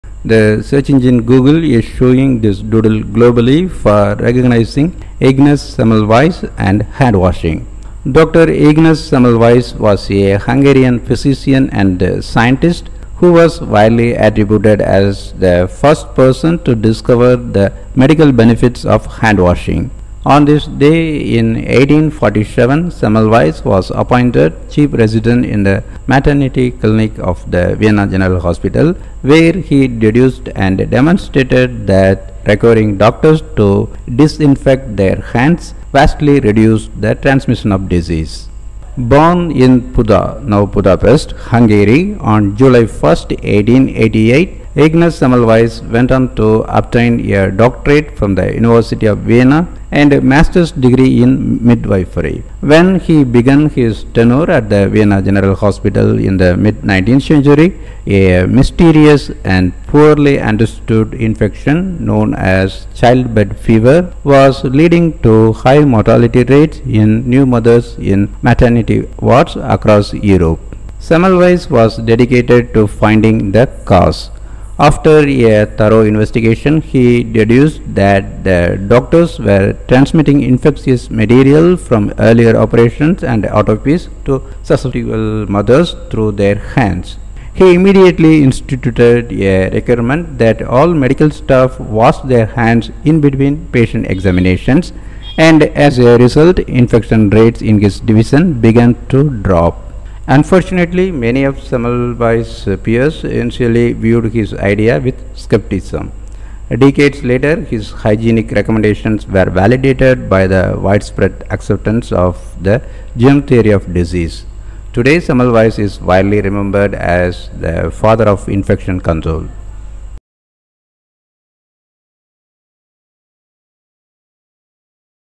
Google shows Doodle for recognizing Ignaz Semmelweis and Handwashing
Google is showing a Doodle globally for recognizing Ignaz Semmelweis and Handwashing.

 Dr. Ignaz Semmelweis was a Hungarian physician and scientist who was widely attributed as the first person to discover the medical benefits of handwashing.

Credit: Google Doodle